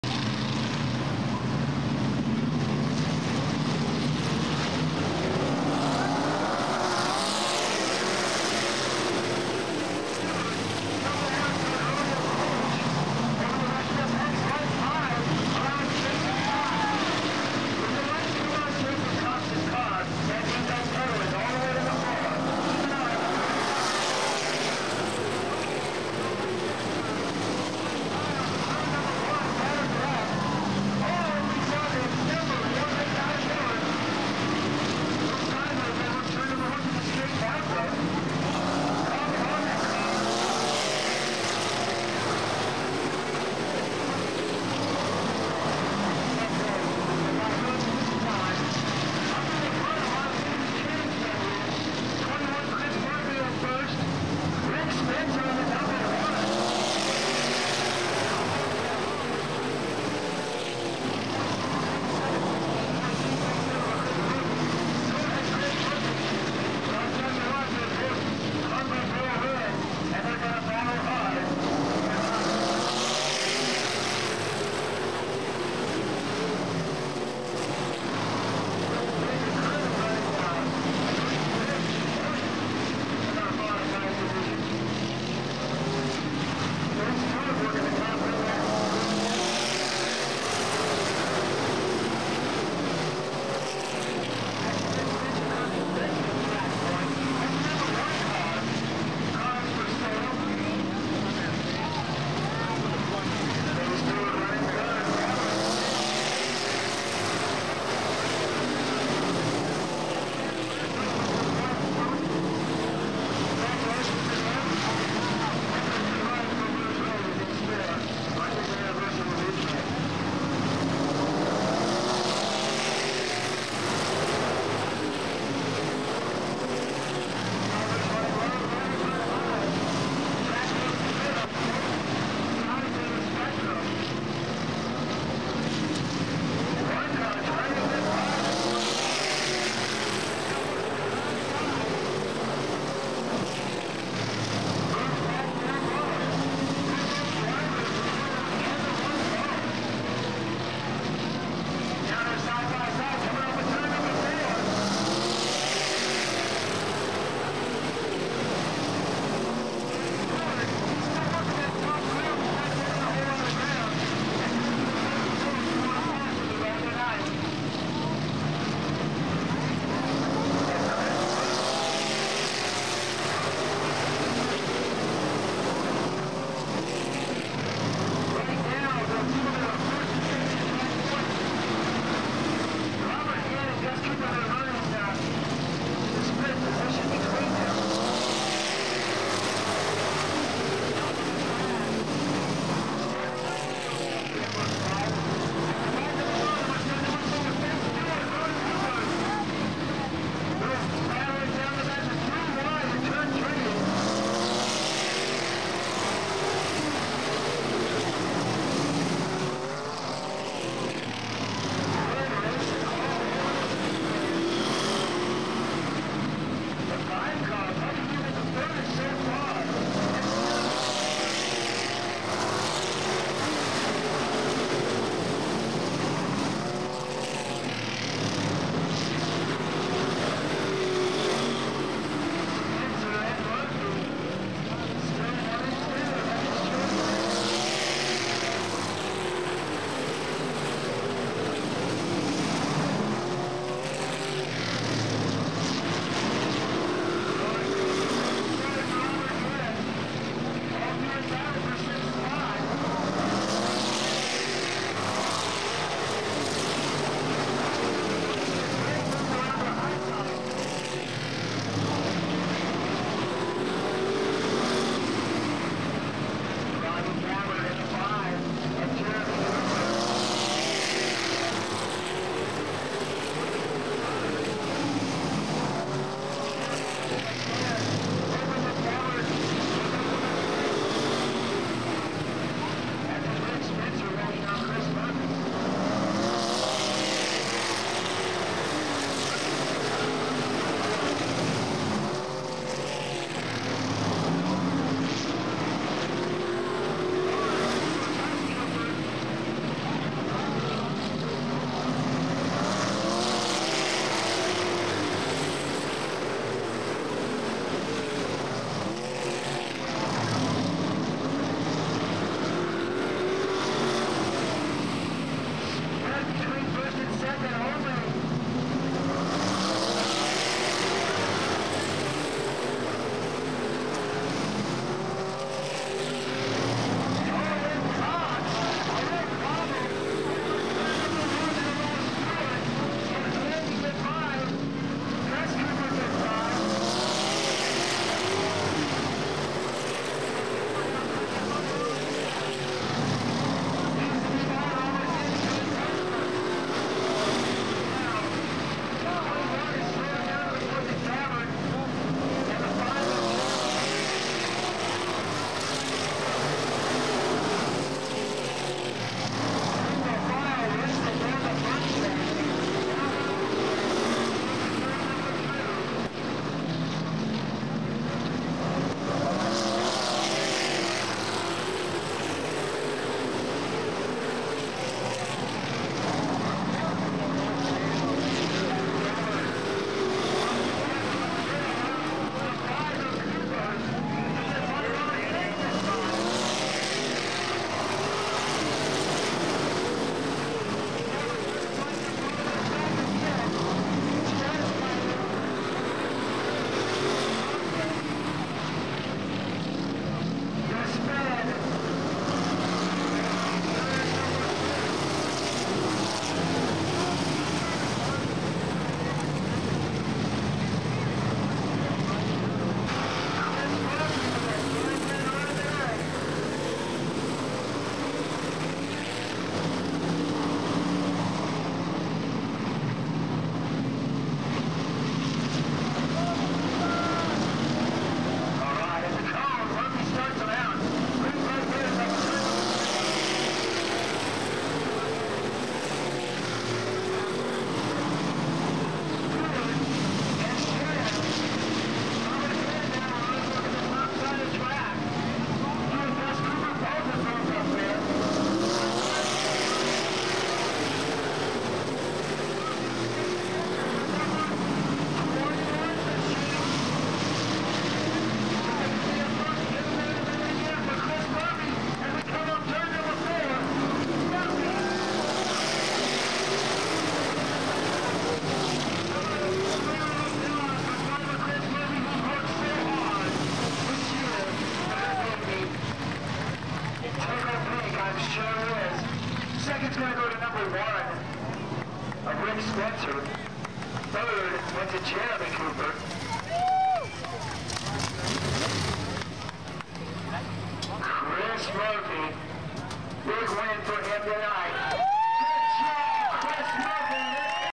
Riverside Speedway Modifed Main Event 8-16-97
Main Event from 8-16-97.  Our first feature win after 10 years.